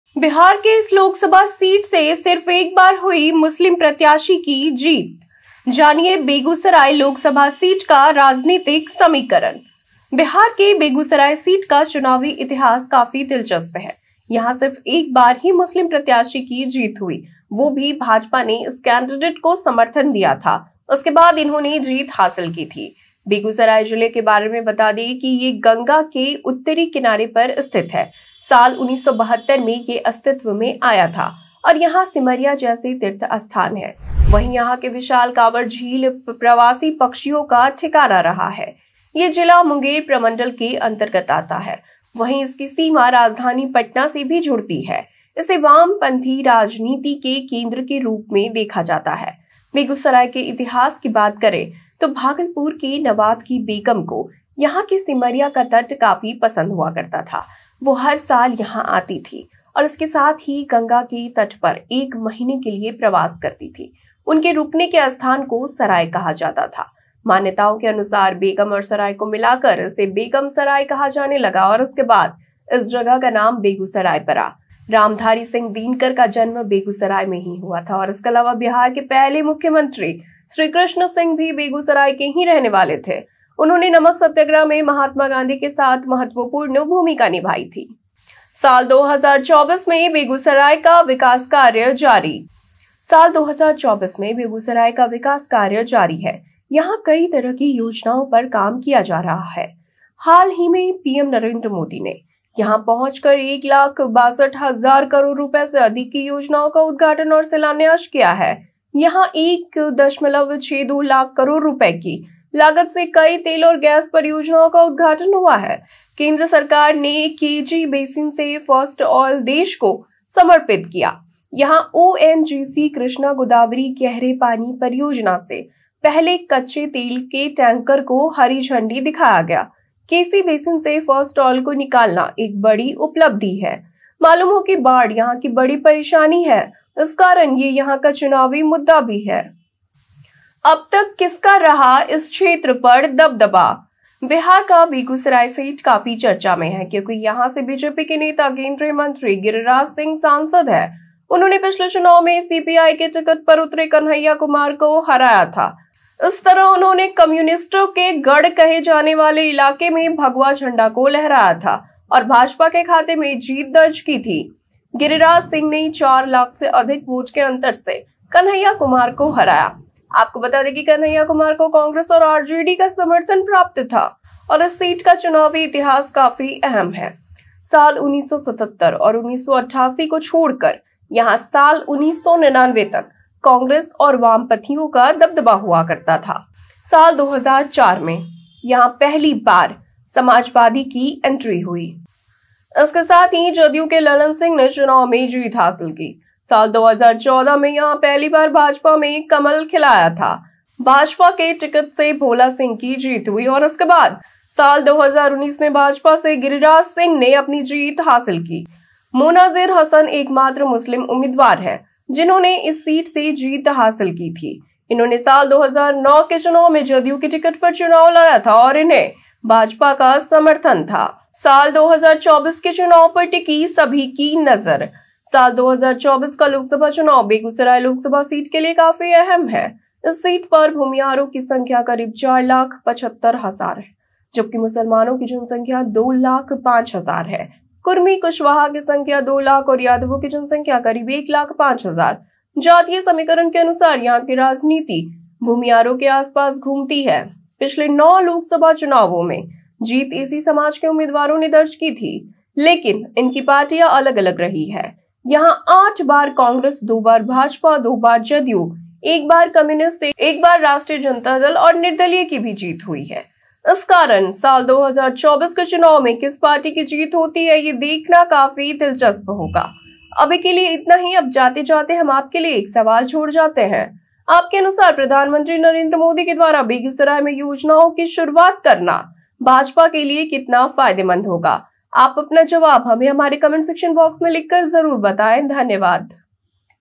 बिहार के इस लोकसभा सीट से सिर्फ एक बार हुई मुस्लिम प्रत्याशी की जीत
बिहार के इस लोकसभा सीट से सिर्फ एक बार हुई मुस्लिम प्रत्याशी की